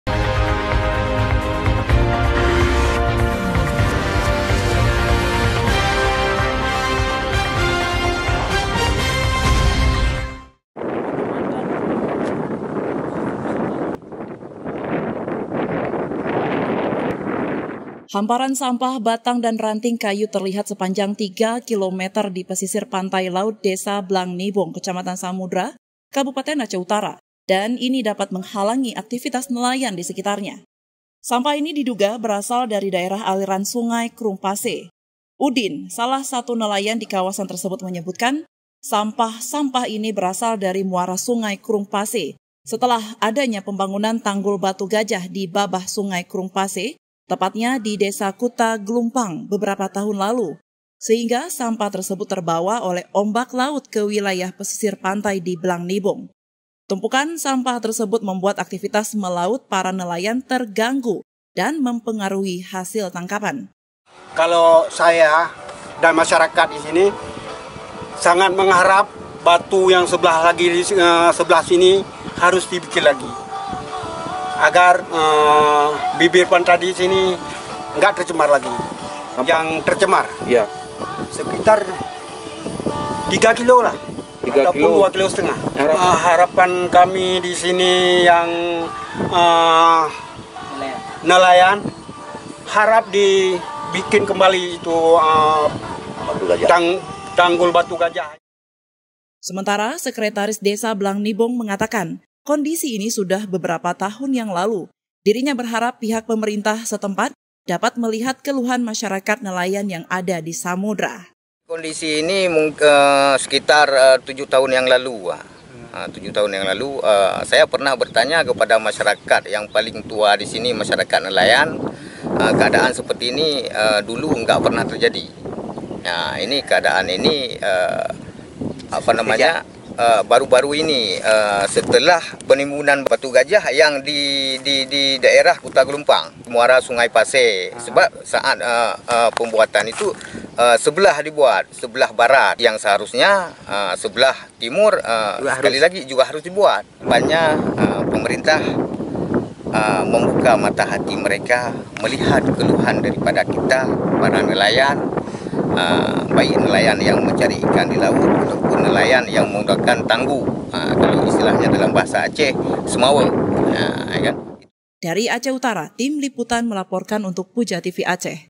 Tumpukan Sampah 3 KM, Selain Jorok Juga Halangi Aktivitas Nelayan Aceh Utara
Nelayan Dan Warga Mengeluh, Pasalnya Kondisi Tumpukan Sampah Di Pinggir Pantai Hingga Saat Ini Belum Ada Langkah Serius Dari Pemerintah Aceh Utara Untuk Menanggulangi Sampah-Sampah  Yang Berasal Dari Muara Sungai Krueng Pasee